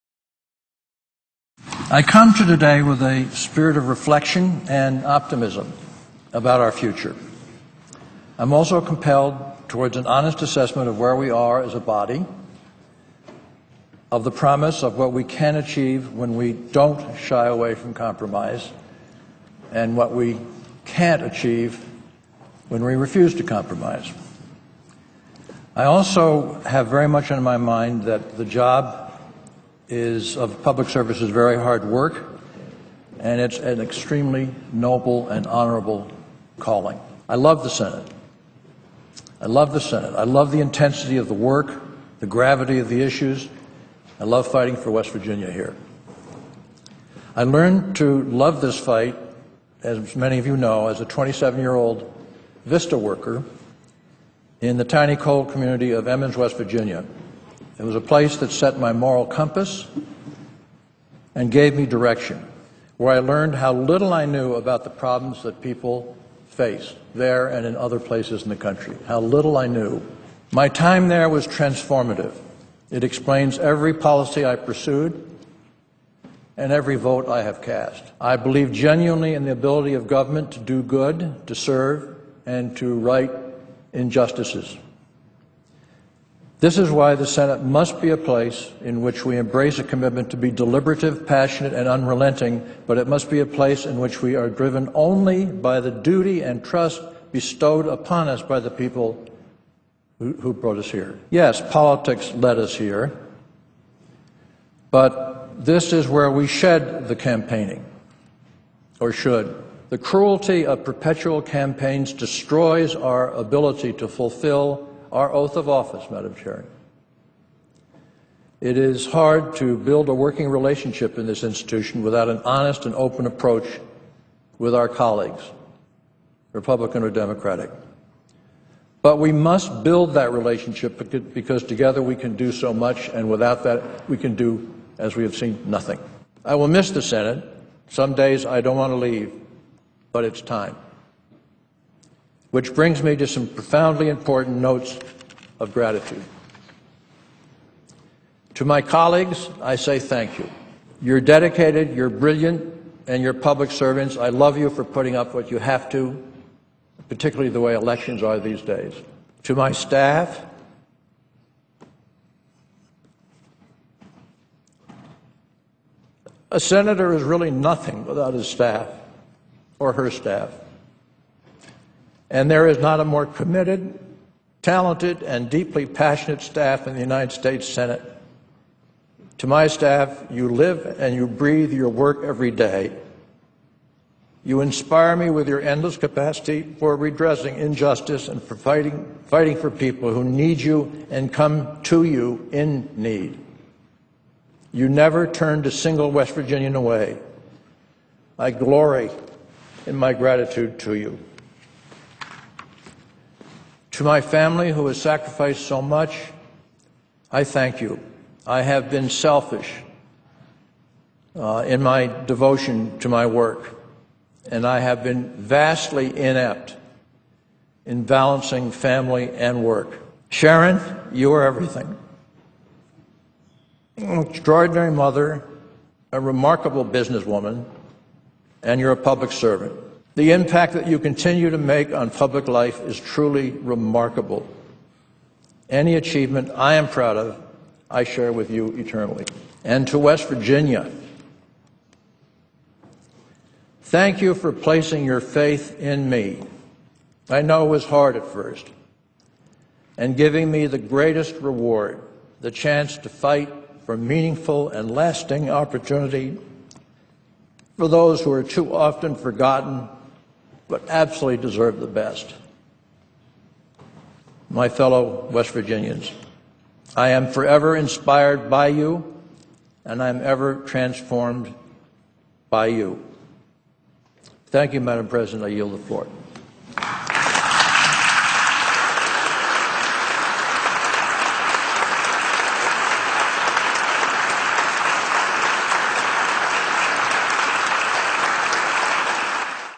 Jay Rockefeller Gives Senate Farewell Address
Jay Rockefeller, the five-term Democratic senator from West Virginia, gave his farewell address on the Senate floor Thursday where he expressed his love for the Senate, warned about the "cruelty of perpetual campaigns" and thanked his family, staff and colleagues.